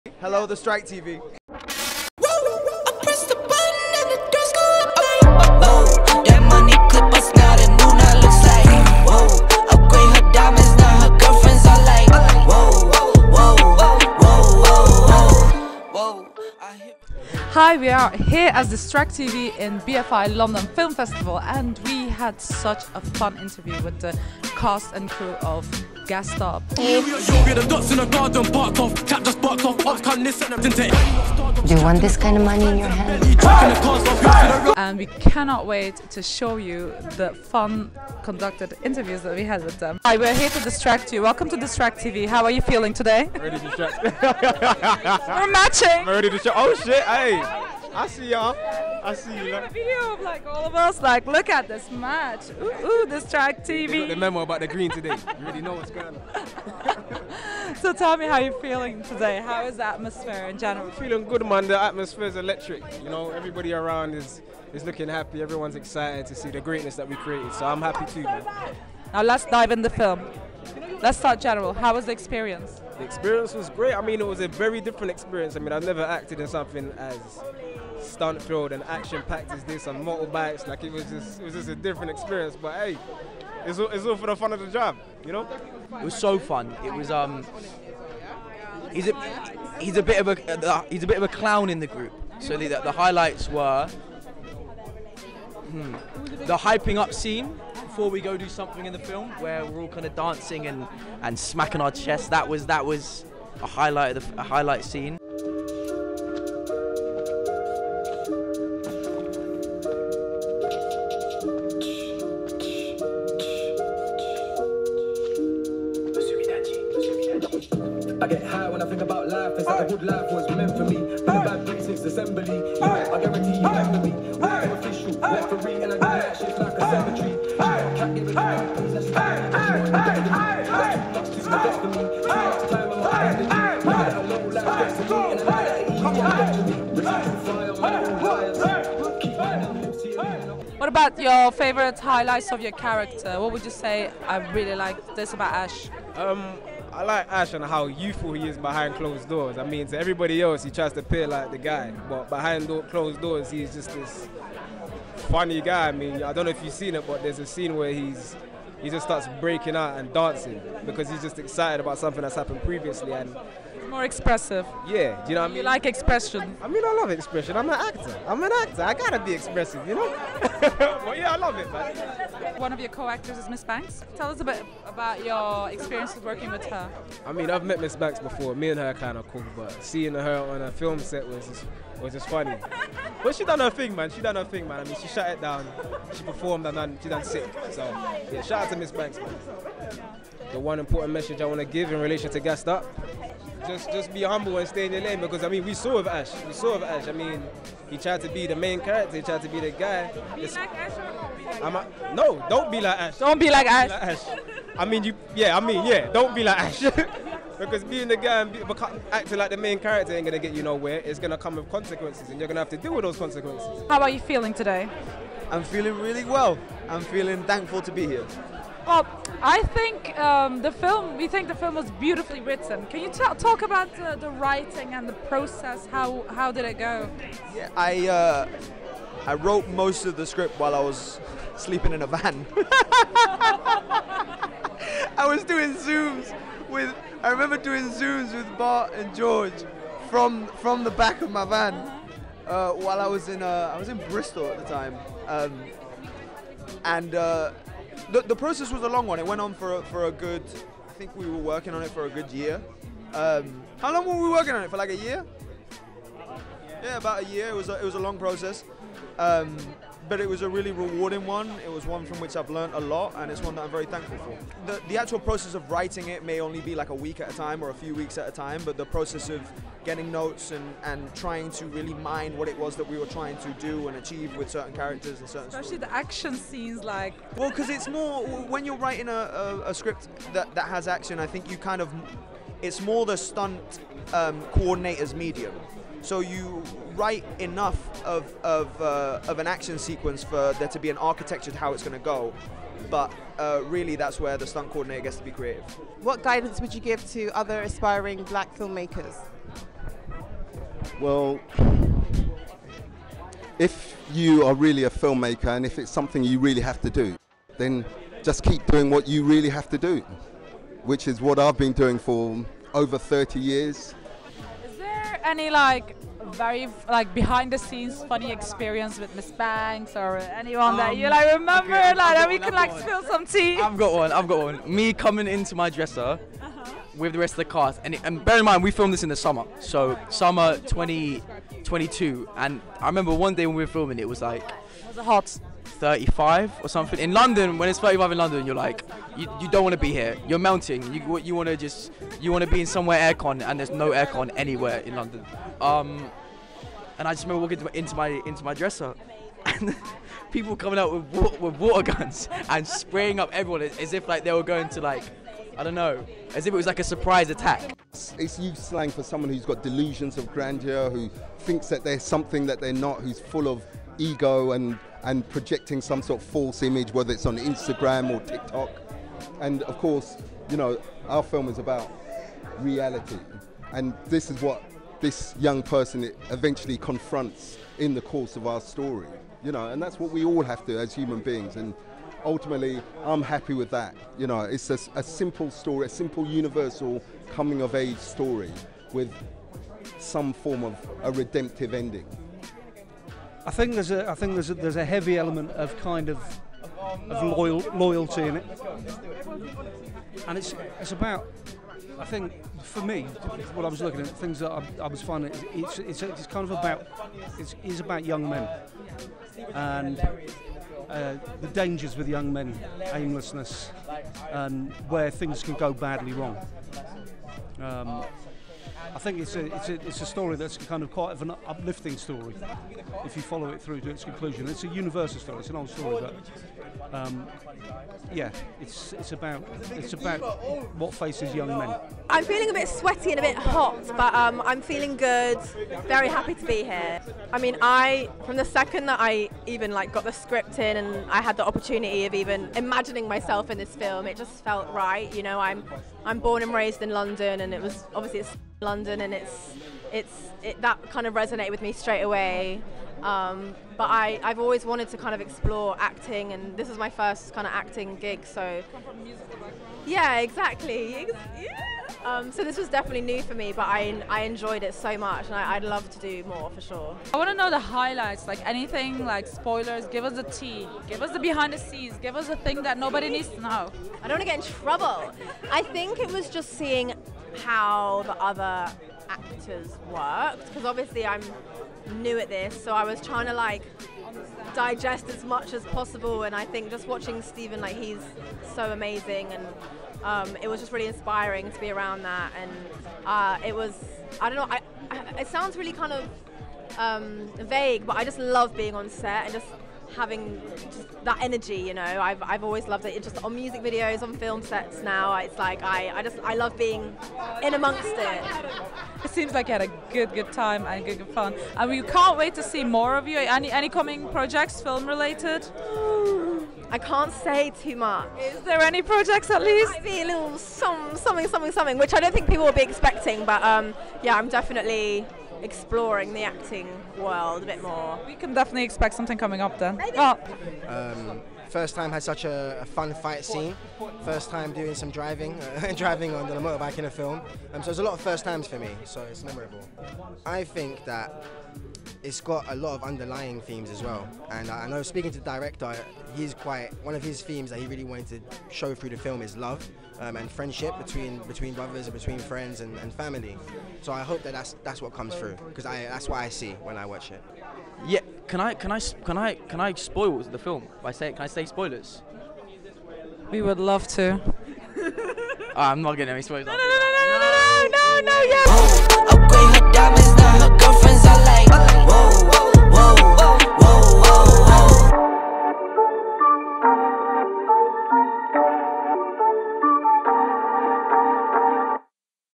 'Gassed Up' Film Premiere at BFI London Film Festival 2023: Exclusive Taz Skylar + Cast Interviews
In this exclusive video, Distract TV takes you to the heart of the BFI London Film Festival for the world premiere of 'Gassed Up' (2023). Join us on the red carpet as we catch up with the film's stellar cast, including Stephen Odubola ('Ash'), Taz Skylar ('Dubz'), and Craige Middleburg, alongside director George Amponsah.

Discover the captivating storyline, the transition of musicians like Mae Muller into the world of acting, and exciting cameos from industry icons like rapper Ms. Banks, Yung Filly, and Harry Pinero. This star-studded event is a testament to the film's unique appeal and the rising stars making their mark in the industry. Don't miss our in-depth interviews, behind-the-scenes insights.

'Gassed Up' is set to be a must-watch, and with stars like Stephen Odubola and Taz Skylar on the rise, the future of cinema looks brighter than ever.

About: Set at the peak of a crime wave sweeping across London, teenage Ash is trying to earn money to raise his 14-year-old sister and send his mother to rehab when he’s caught up with an organised crime ring led by a young and charismatic Albanian immigrant. As the crimes escalate from stealing mobile phones on the street to full on jewellery store robberies, Ash must choose between his lavish new lifestyle or the people he loves before his whole world comes crashing down around him.

Director: George Amponsah

Cast: Stephen Odubola, Taz Skylar, Mae Muller, Steve Toussaint, Harry Pinero, Yung Filly, Ms Banks
Craige Middleburg, Jelena Gavrilovic

Released in cinemas from: 9th Feb 2024

Main Host: Ceren Ocal
Producer: Sharon Ayiglo
with the cast & director of Gassed Up (2023)

Editor: Sharon Studios
Location: BFI Southbank

Camera Operator: Omer Dernek
Additional Camera: Sharon Ayiglo
Sound: Ceren Ocal

Additional Footage Courtesy of BFI London Film Festival